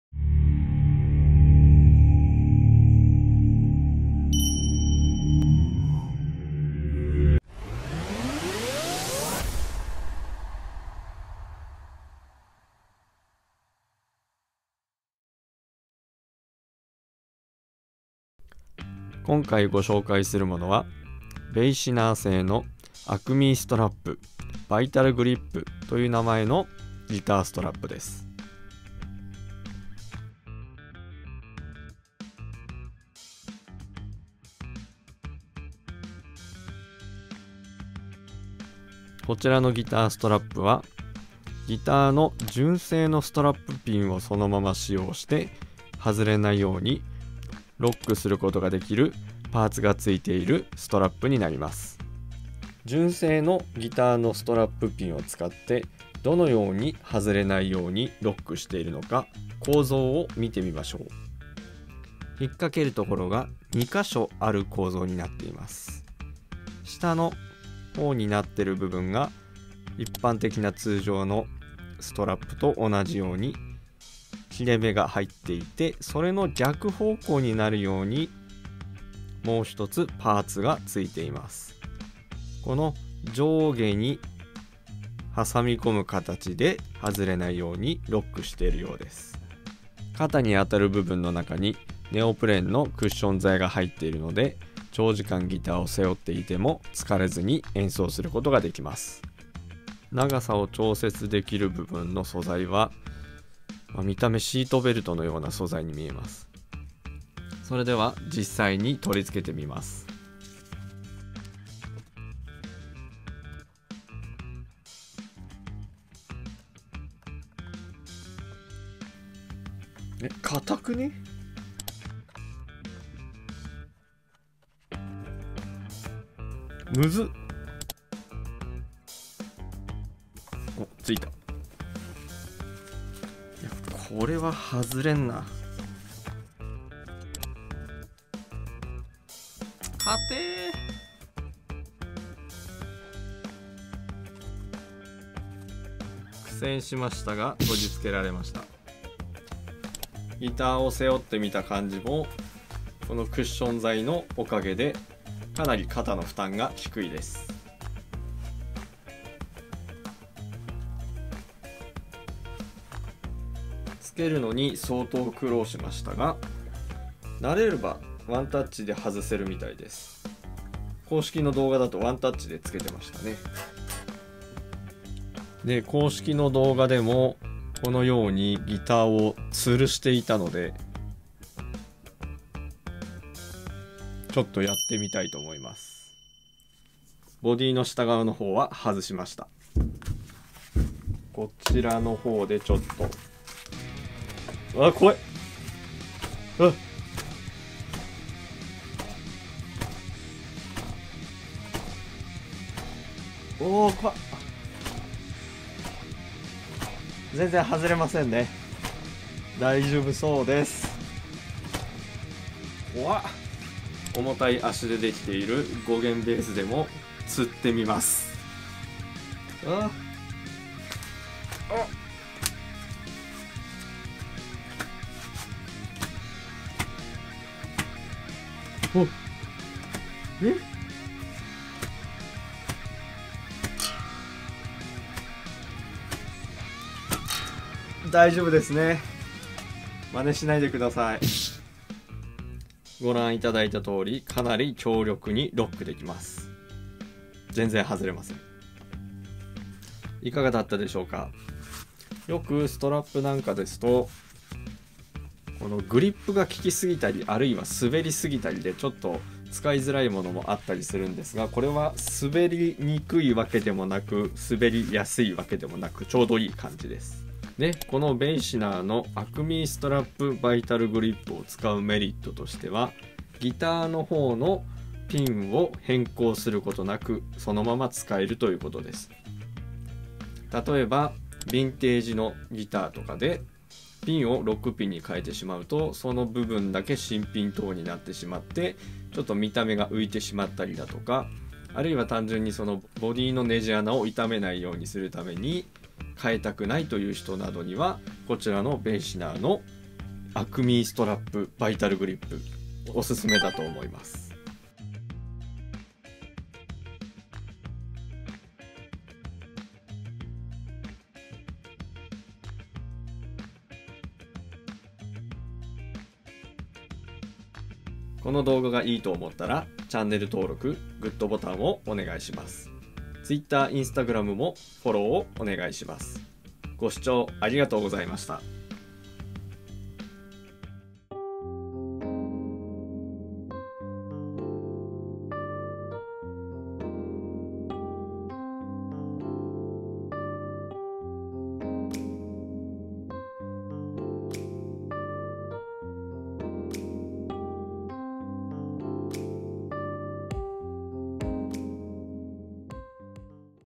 純正ピンなのに外れないギターストラップ！basiner (ベイシナー)ACME STRAP VITALGRIP
ロックピンに変更しなくても外れない構造のギターストラップがあった！٩(๑òωó๑)۶！
basiner(ベイシナー) 日本語ホームページ

金子レフティ裕亮
H_LEFTY_K